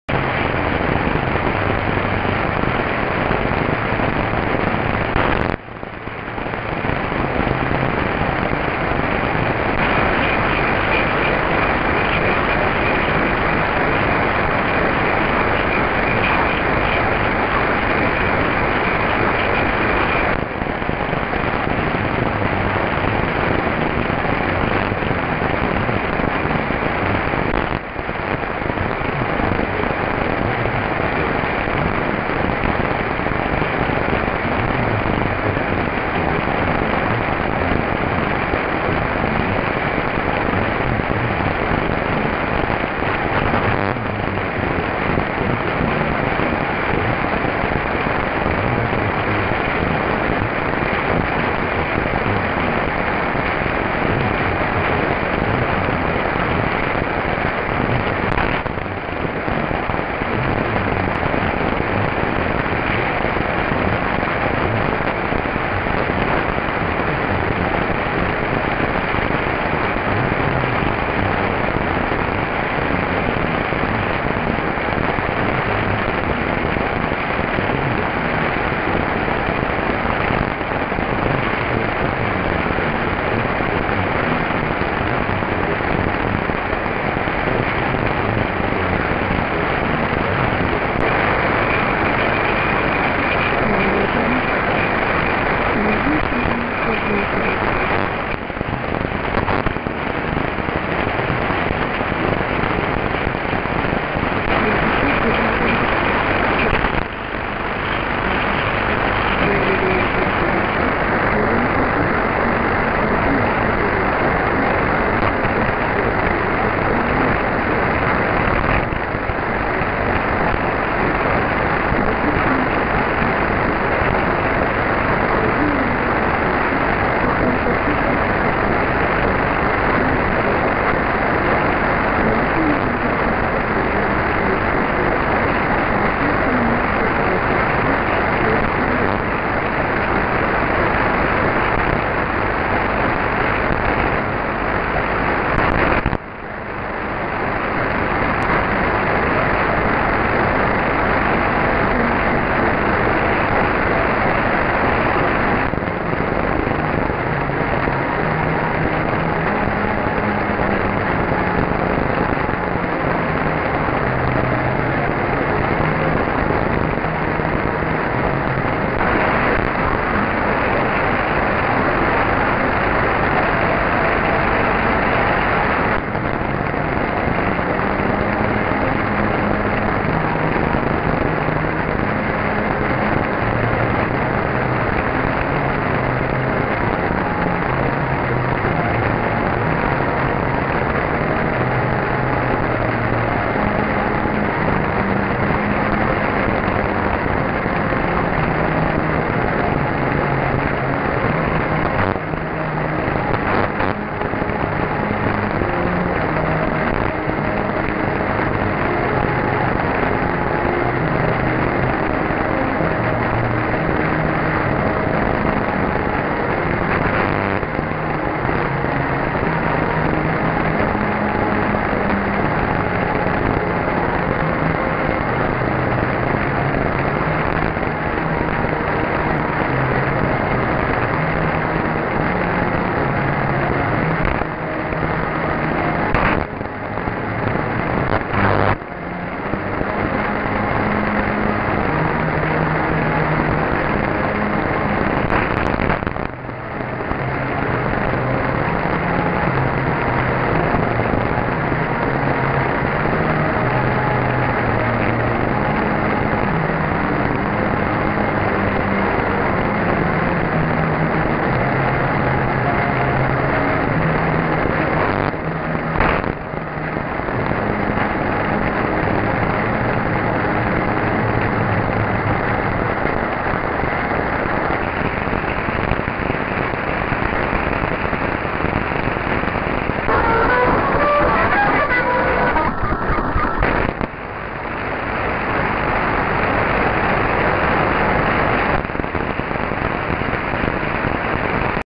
164 & 209 kHz - Mongolia?
Weak signal, need headphones!
QTH: Székesfehérvár, Hungary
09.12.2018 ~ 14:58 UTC